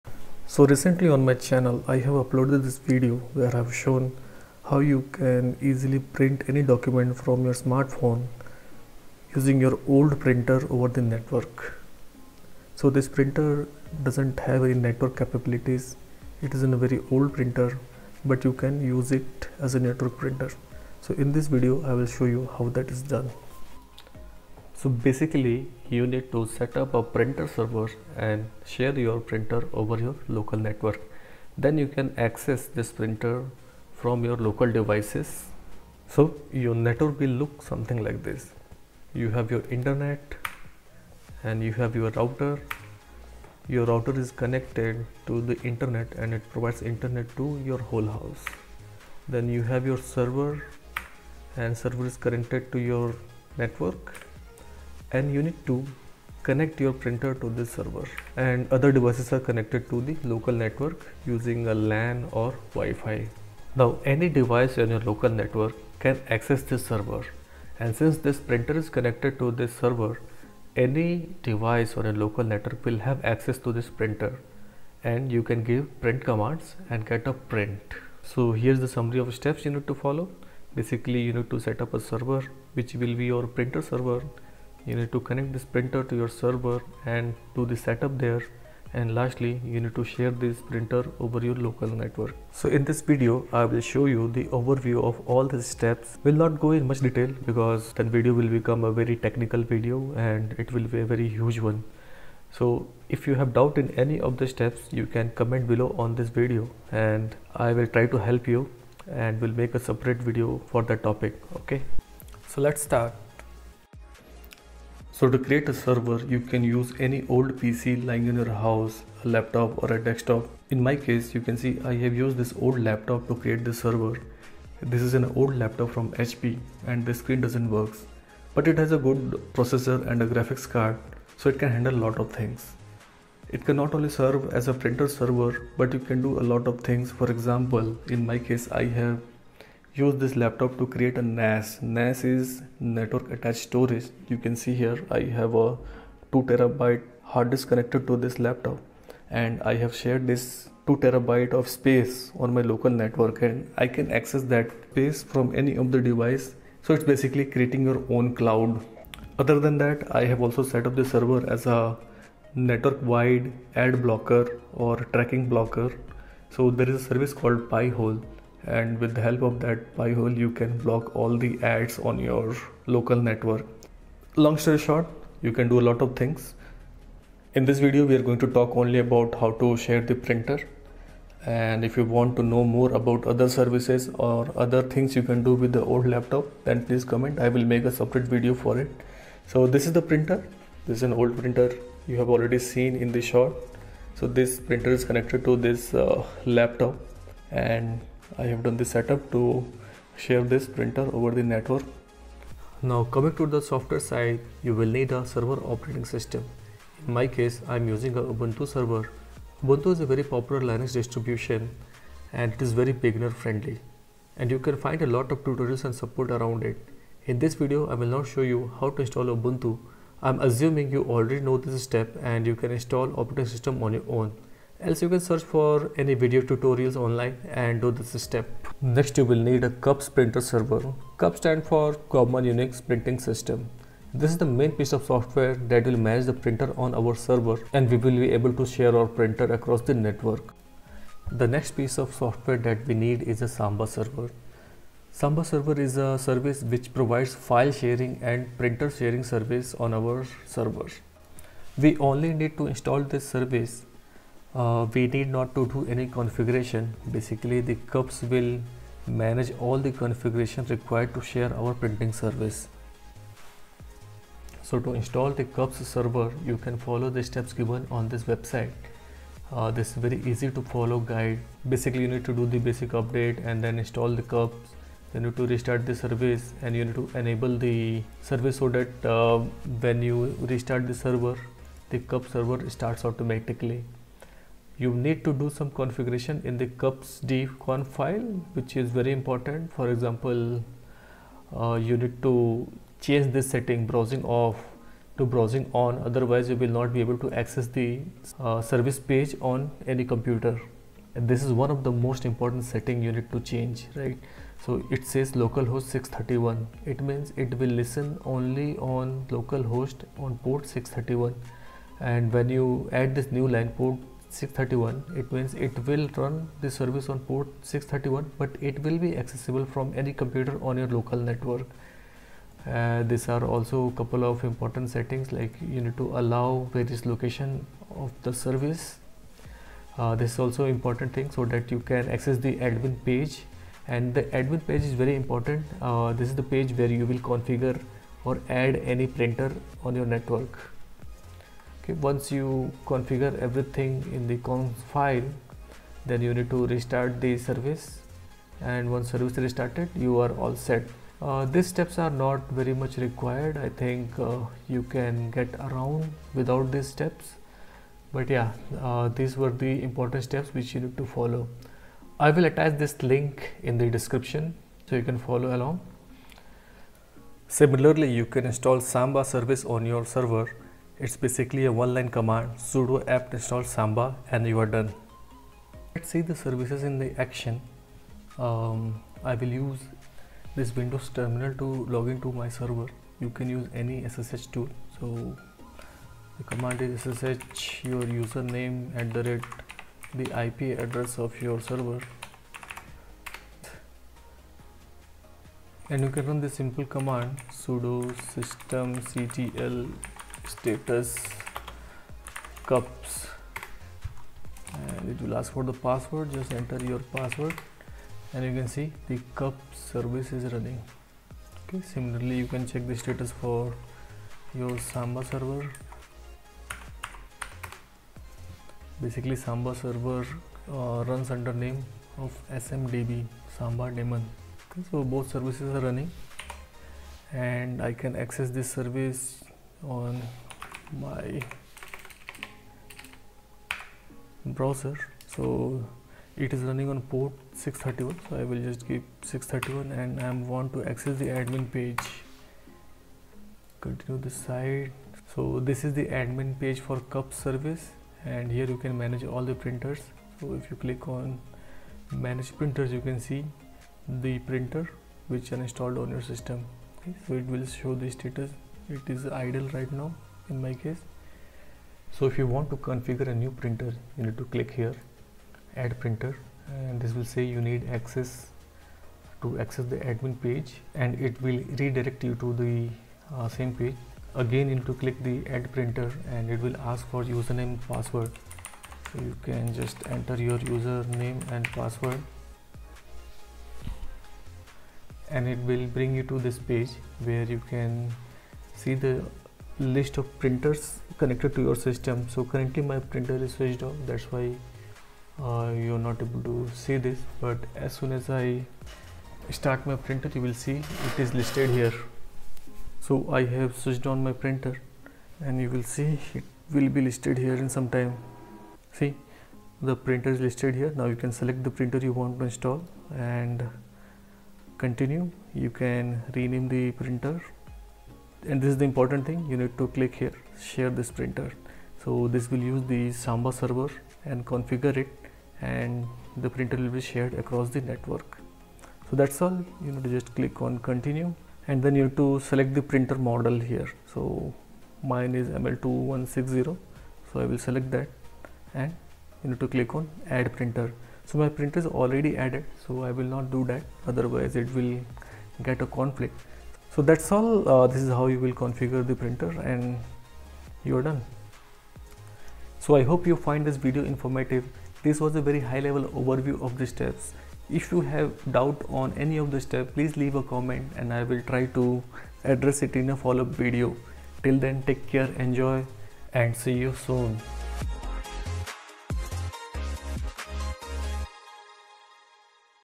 How to convert any old USB Printer into WIFI/Network printer | Print from Phone
Hey there! If you happen to own an older USB printer and are interested in transforming it into a wireless printer, you're in the right place. Be sure to stick around until the end of the video to catch all the crucial steps. In today's tutorial, I'll guide you through the process of converting any USB printer into a wireless one.